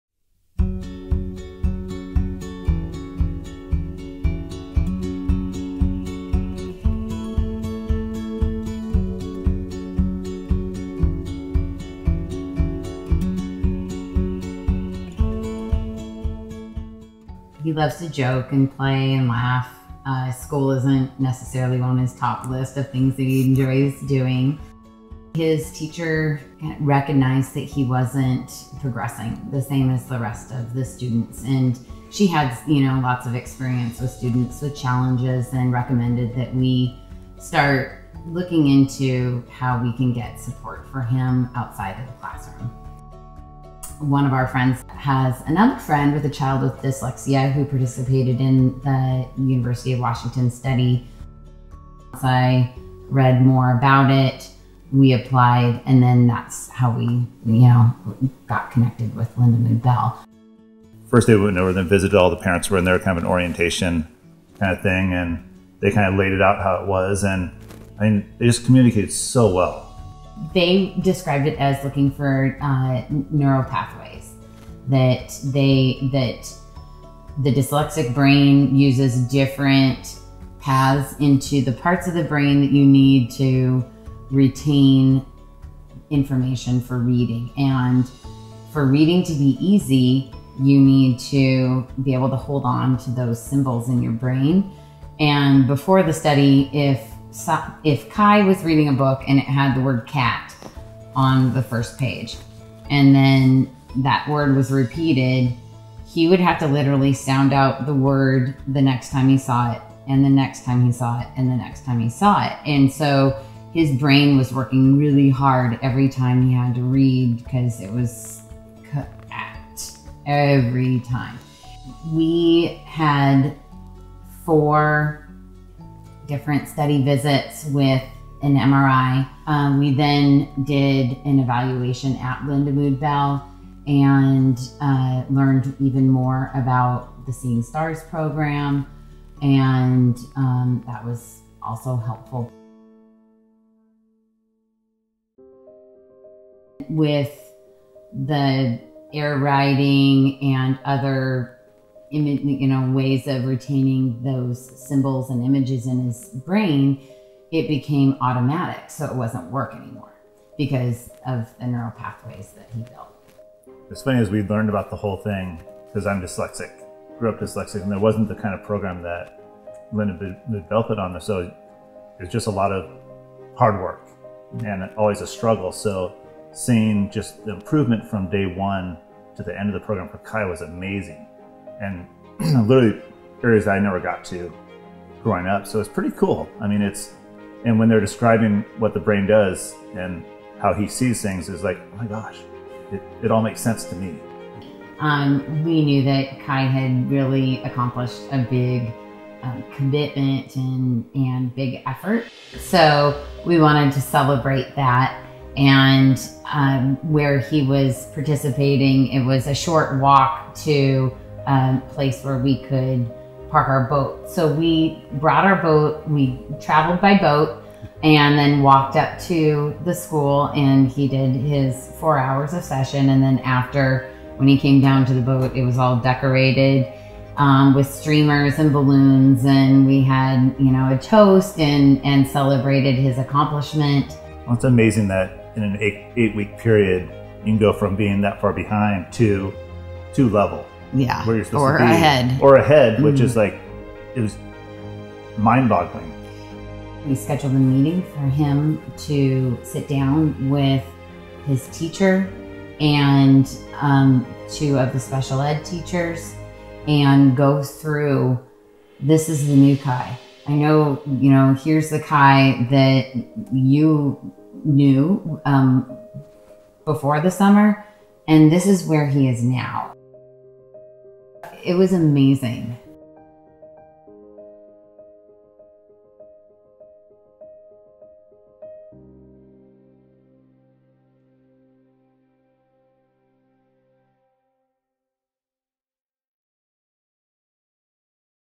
Research study on dyslexia changes life of participant | Lindamood-Bell Learning Centers
The Institute for Learning & Brain Sciences at the University of Washington conducted a study examining growth in reading skills and white matter as a result of intensive reading intervention. MRI data collected during instruction indicates that there were large-scale changes in white matter correlating with the gains in reading. Further, the study identifies white matter tracts that may predict the ease with which a child learns how to read.

In this video, parents Dan and Kelly describe their son’s learning and the experience of having him participate in the study.